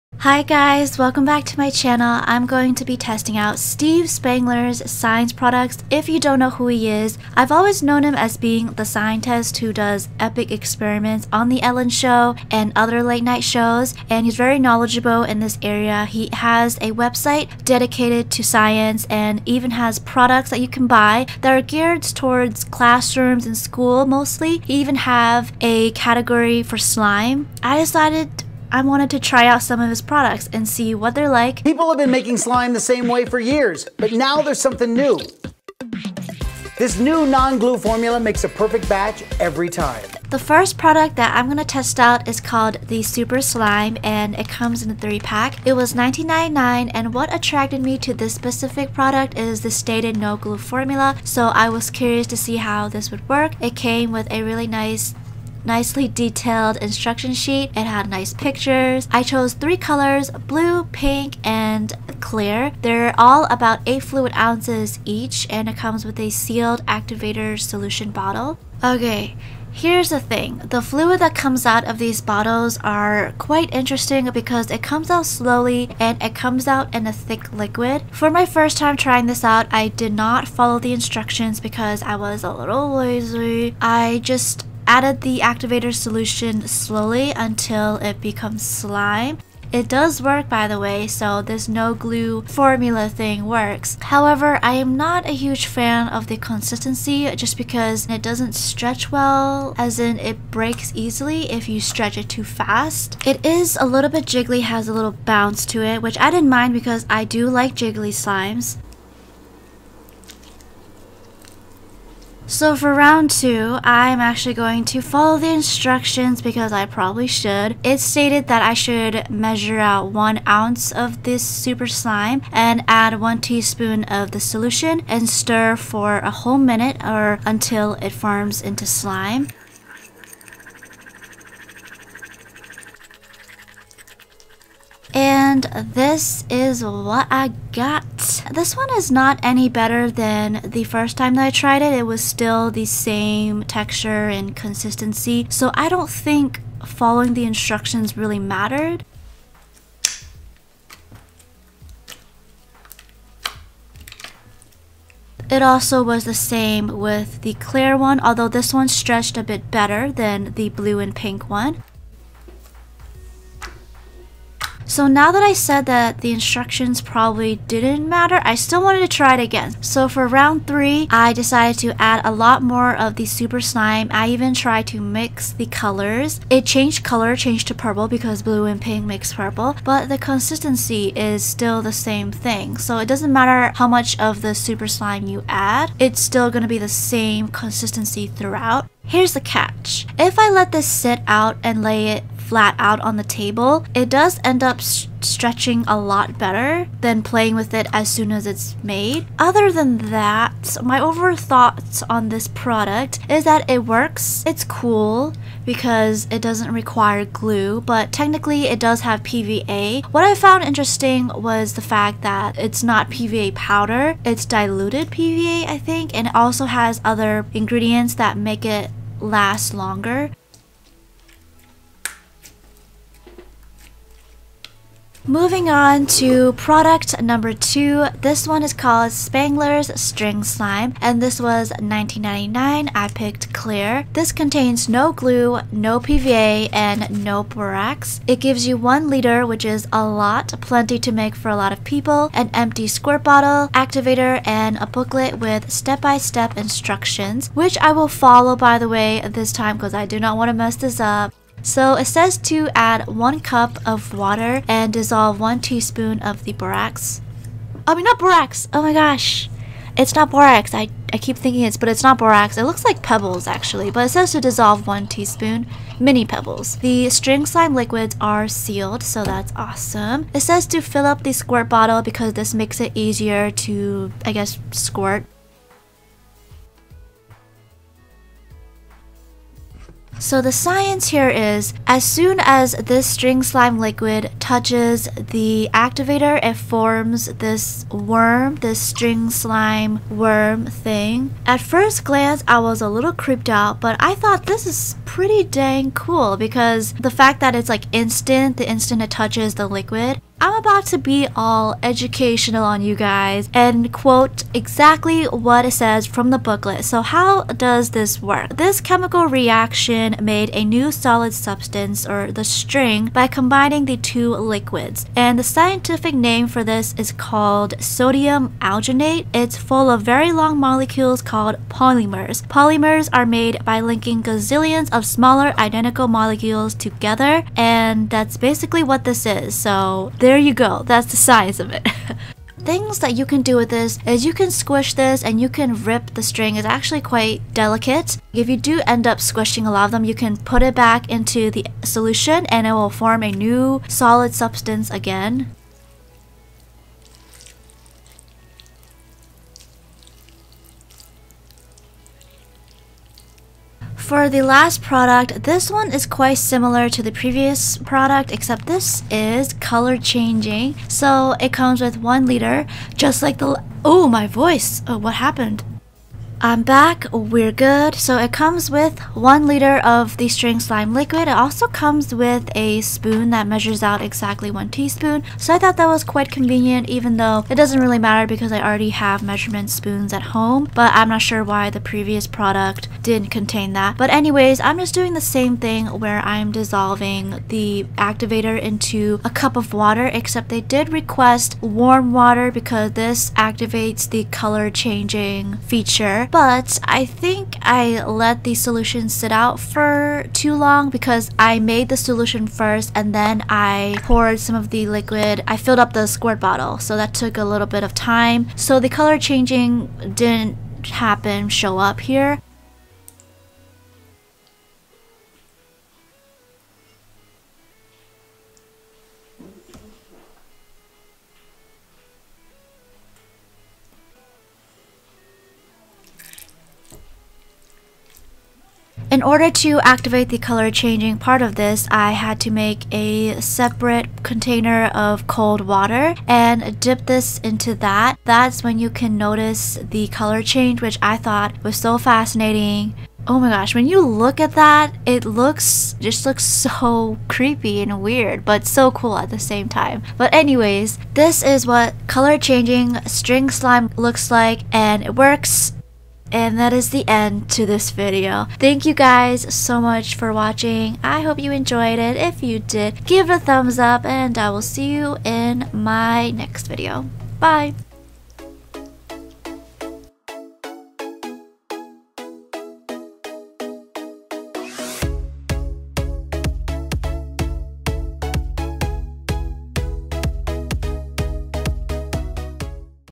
Testing Steve Spangler's No Glue Slime Products
➳ Hello everyone!

Hi guys! I'm testing Steve Spangler's slime/science products in this video. Hope you enjoy!

Products mentioned:
Spangler's String Slime

➳ Previous video:
Homemade Glue vs. Store Bought Glue! Can Homemade Glue make Slime?